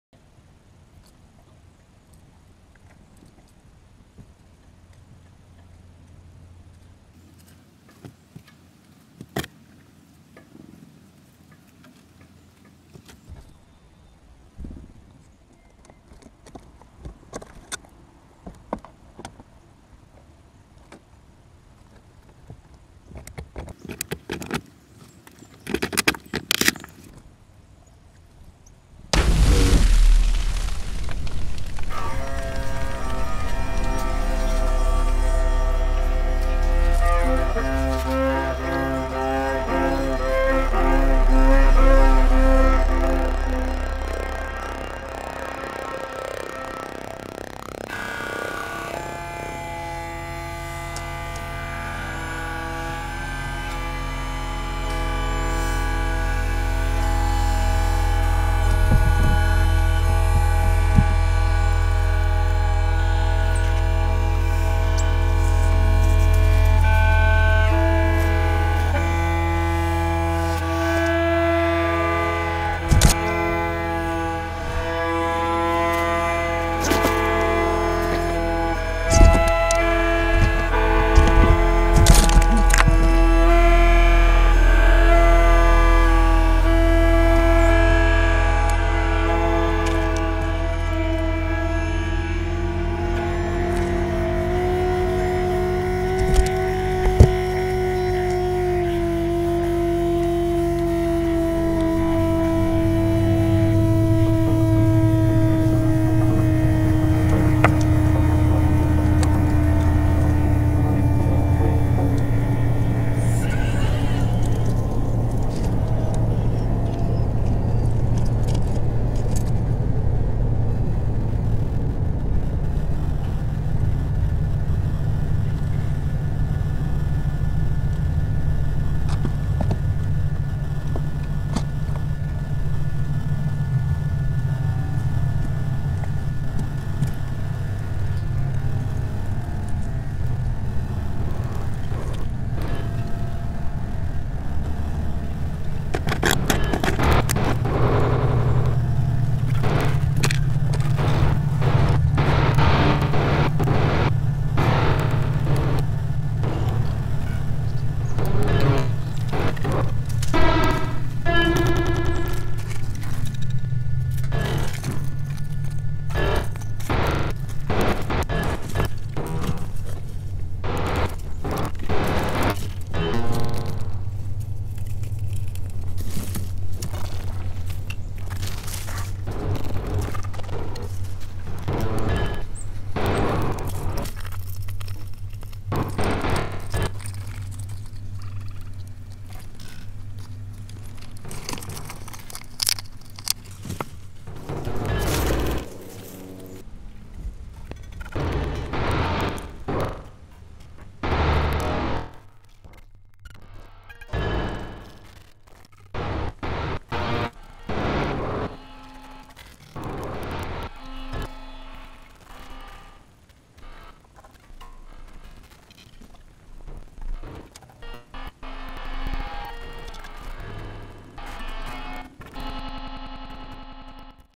Chipmunk & Goliath (the grey squirrel)
Some avant-garde action. Trying to capture the suspense and danger of a small animal entering a small house with one entrance/exit surrounded by larger animals who want to enter the same house.
Music from FMA free music archive by Jaan Patterson